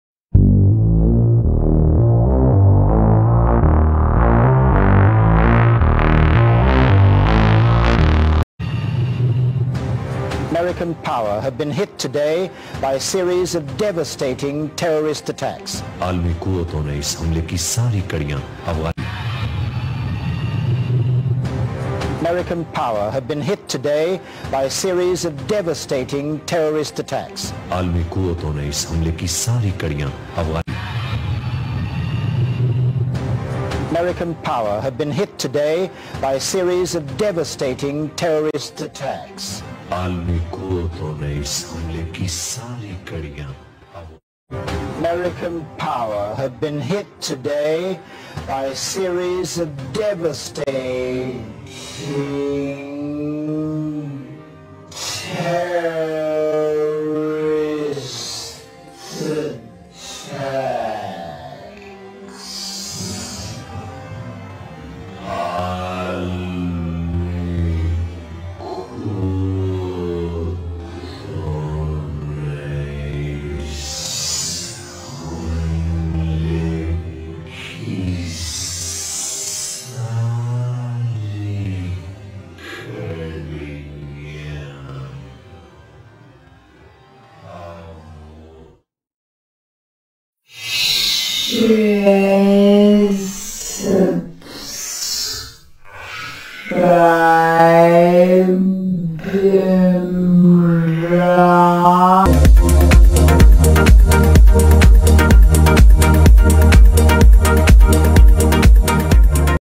Shahid Afridi caught chewing tobacco Naswar on live TV, gets trolled online
Former Pakistan cricketer Shahid Afridi has found himself amid a controversy after he was caught on live television chewing tobacco at the Defence Day ceremony.
The event was held at the General Headquarters in Rawalpindi.
Mr. Boom Boom was reportedly chewing Naswar when a rolling camera stopped at him and captured him in the act.
Ever since the video was shared on Twitter, people haven't stopped trolling him.